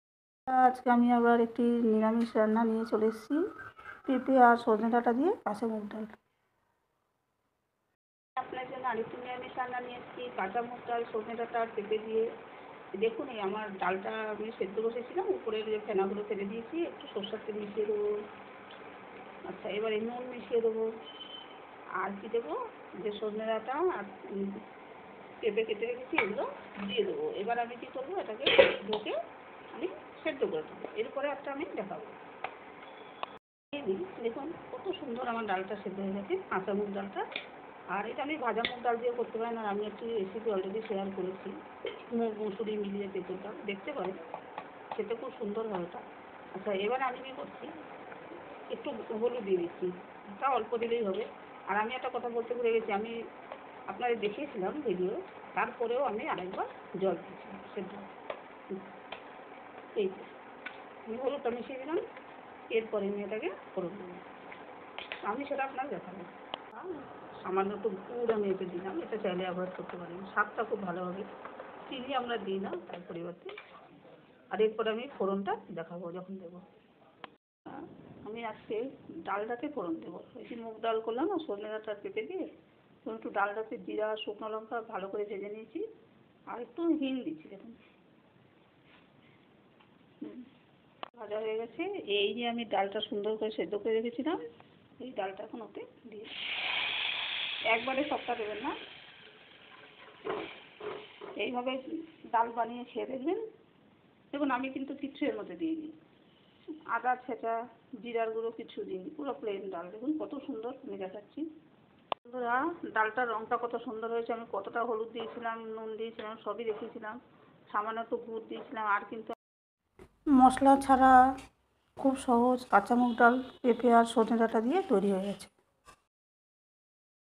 সজনেডাঁটা ও পেঁপে দিয়ে কাঁচা মুগ ডাল /moong dal recipe
সজনেডাঁটা ও পেঁপে দিয়ে কাঁচা মুগ ডাল @chandraguhacuisineworld5714
ingredients :-raw moong dal, sliced papaya and drumsticks, Salt, turmeric powder, jaggery (optional) water, raw mustard oil,
mustard oil cumin seeds dry red chilli hing